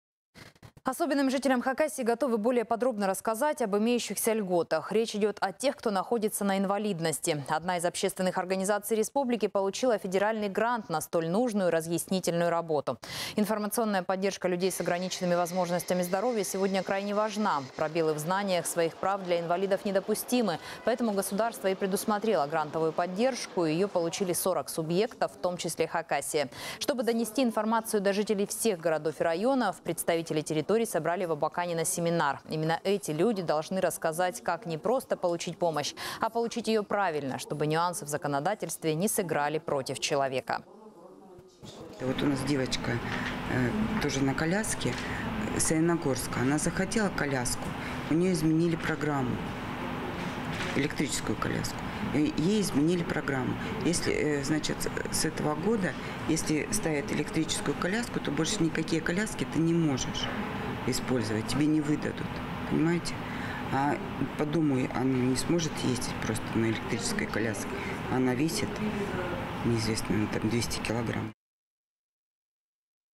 Особенным жителям Хакасии более подробно расскажут об имеющихся льготах
Люди с ограниченными возможностями здоровья должны иметь полную информацию о льготах и гарантиях, которые дает им государство. Одна из общественных организаций республики получила федеральный грант на такую нужную разъяснительную работу.